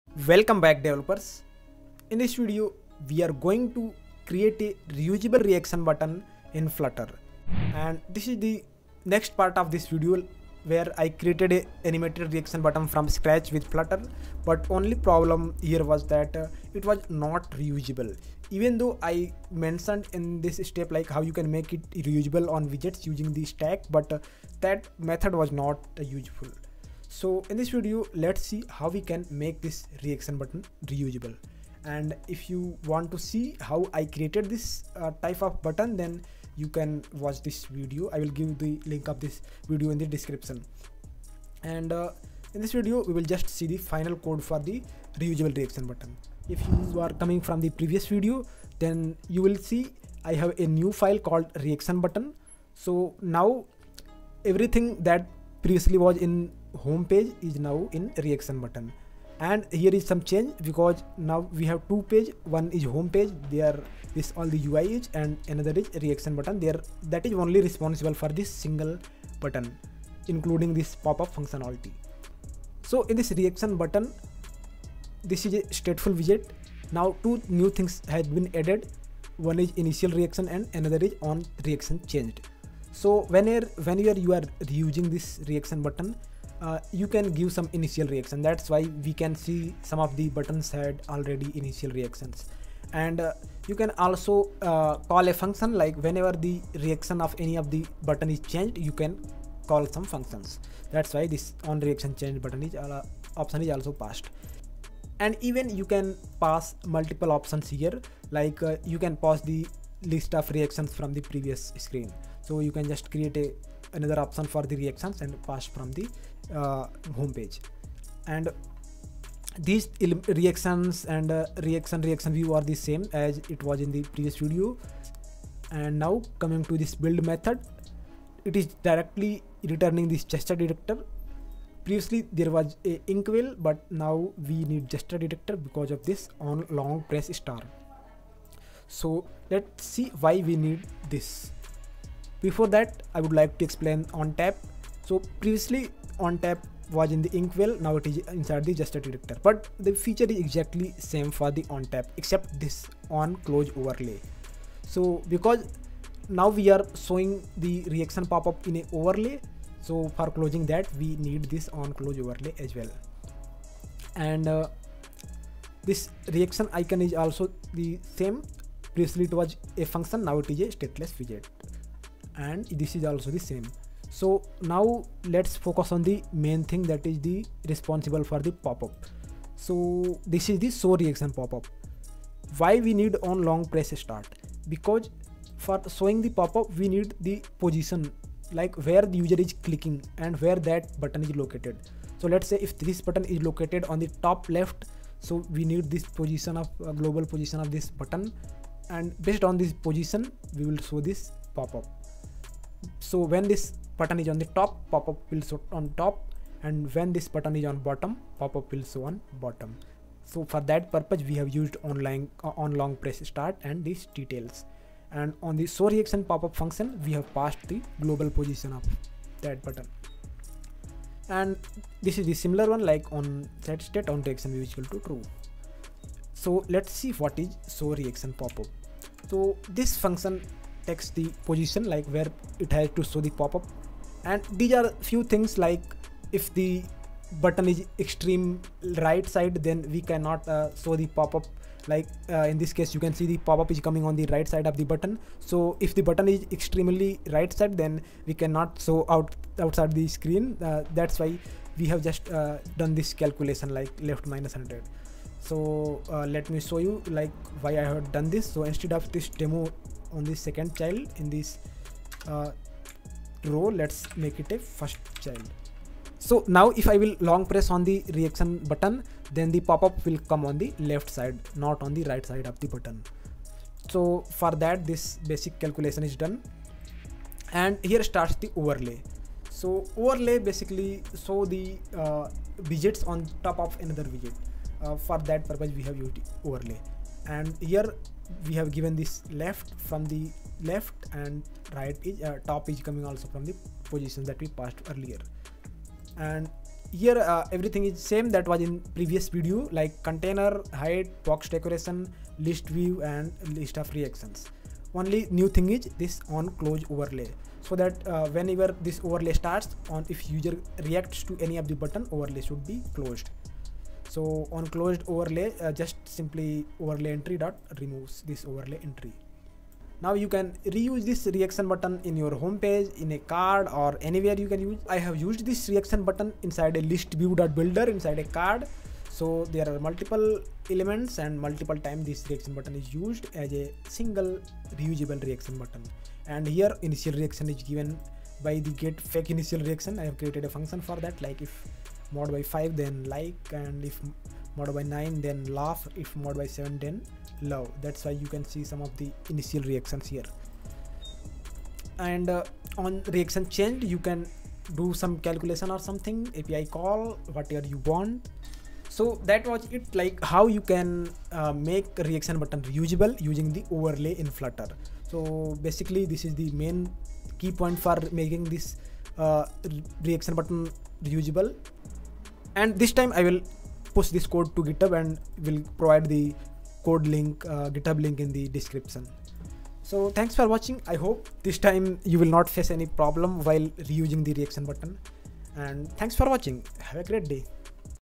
Create a Reusable Reaction Button with Flutter - Source Code Available
In this video I have explained how you can develop a reusable reaction button with flutter.

This is the part 2 of the video where I have explained how to make button reusable.
If you want to see it from scratch like how to design it watch this video -